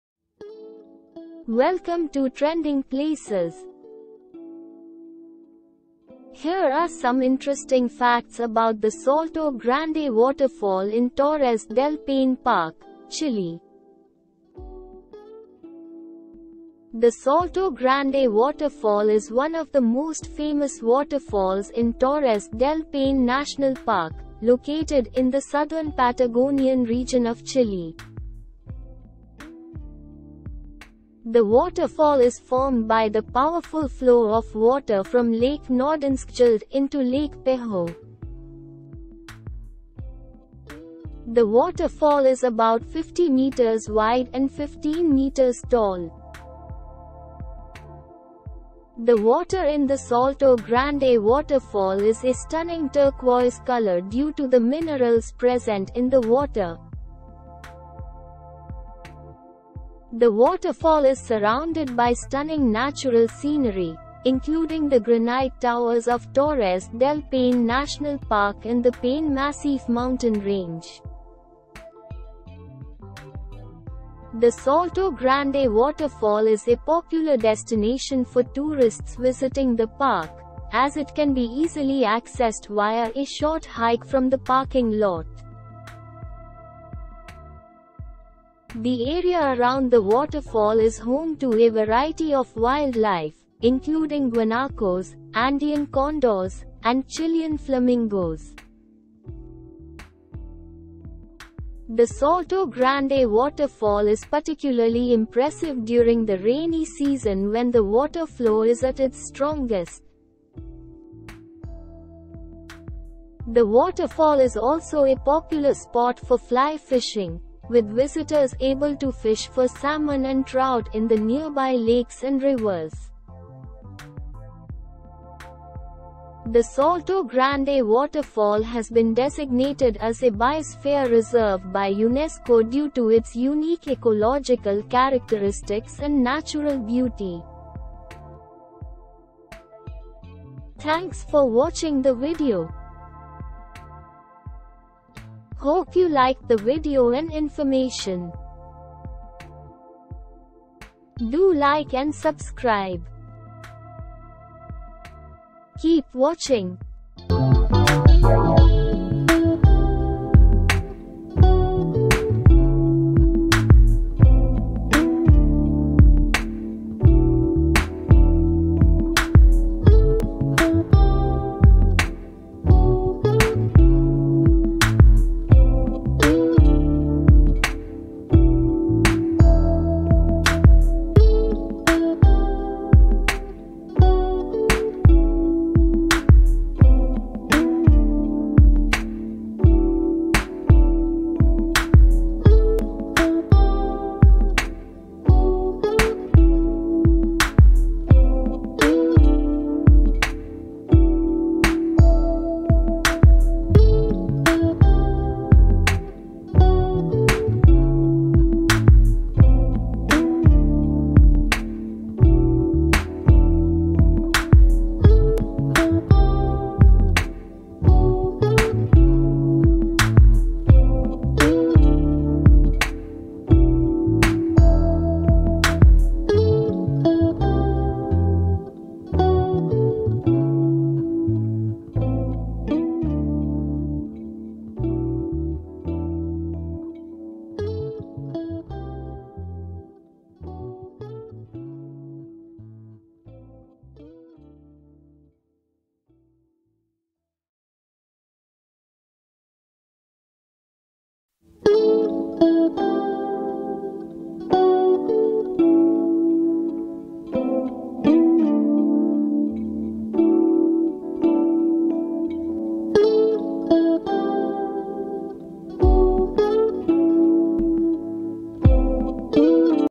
Salto Grande Waterfall in Torres Del Paine Park, Chile. Trending Places to see in 2023
The Salto Grande Waterfall is one of the most famous waterfalls in Torres del Paine National Park, located in the southern Patagonian region of Chile.

The waterfall is formed by the powerful flow of water from Lake Nordenskjöld into Lake Pehoe.

The waterfall is about 50 meters (164 feet) wide and 15 meters (49 feet) tall.

The water in the Salto Grande Waterfall is a stunning turquoise color due to the minerals present in the water.

The waterfall is surrounded by stunning natural scenery, including the granite towers of Torres del Paine National Park and the Paine Massif mountain range.

The Salto Grande Waterfall is a popular destination for tourists visiting the park, as it can be easily accessed via a short hike from the parking lot.

The area around the waterfall is home to a variety of wildlife, including guanacos, Andean condors, and Chilean flamingos.

The Salto Grande Waterfall is particularly impressive during the rainy season when the water flow is at its strongest.

The waterfall is also a popular spot for fly fishing, with visitors able to fish for salmon and trout in the nearby lakes and rivers.

The Salto Grande Waterfall has been designated as a Biosphere Reserve by UNESCO due to its unique ecological characteristics and natural beauty.